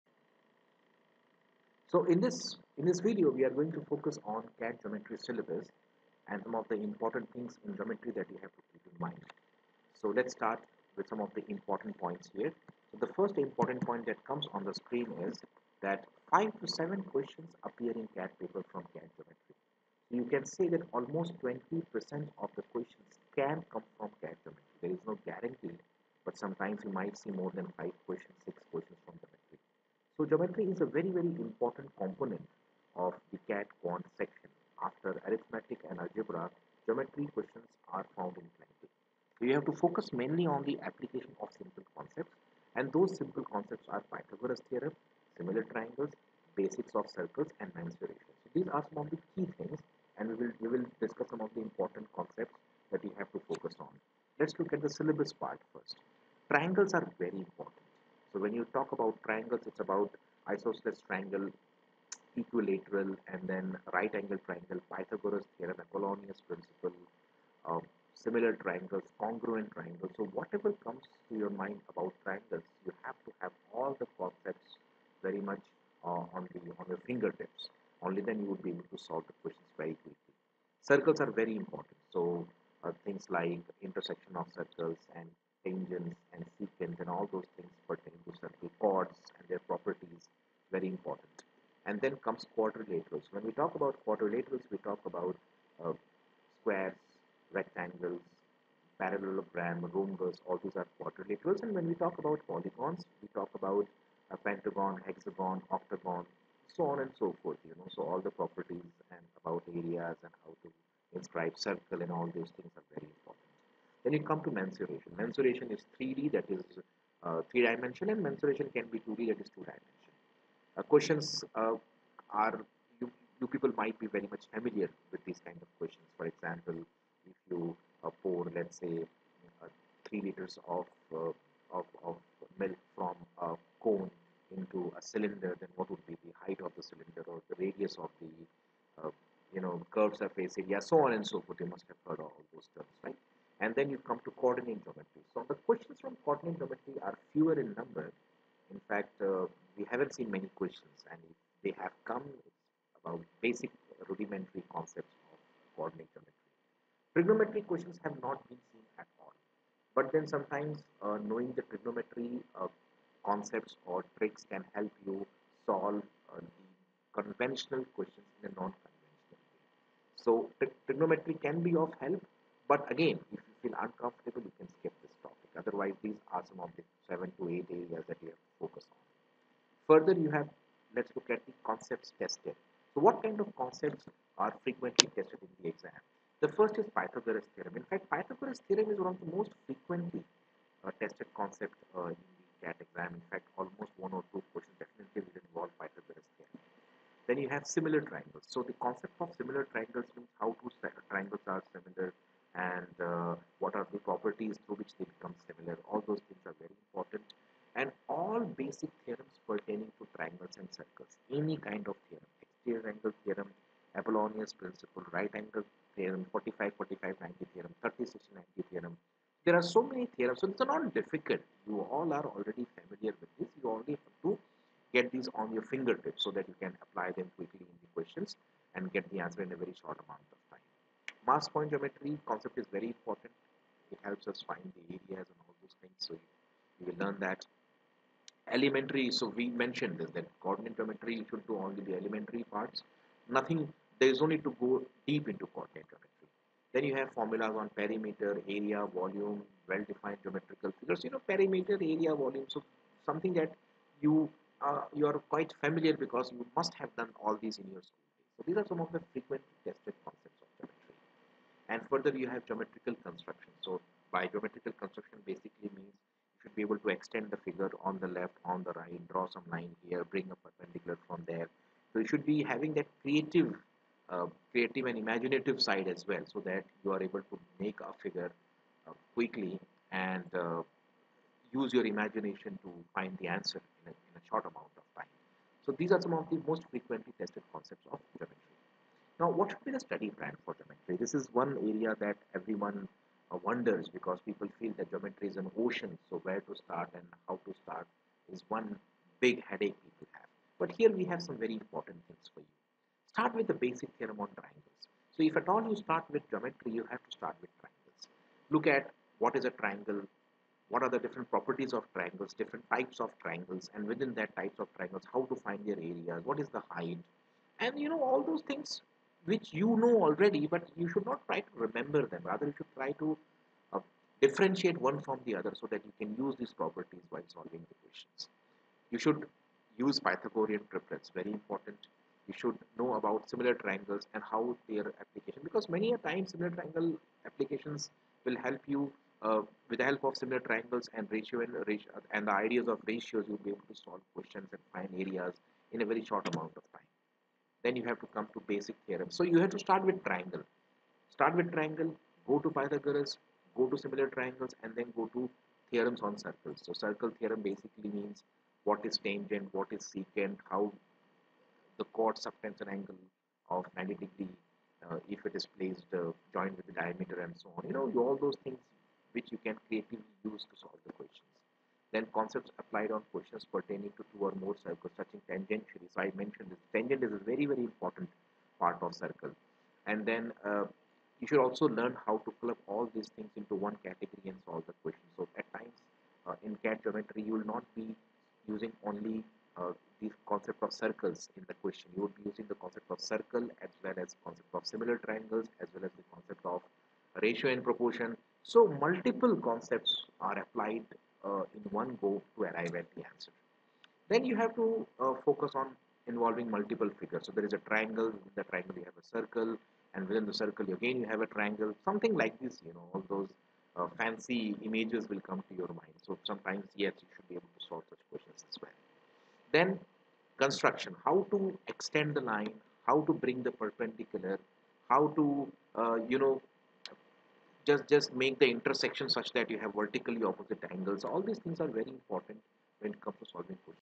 CAT Geometry Syllabus
In this Video we have listed syllabus with the important topics from Geometry for CAT.
CAT Online Courses:
2. CAT VARC online Course
3. CAT Quant online Course
4. CAT DILR online Course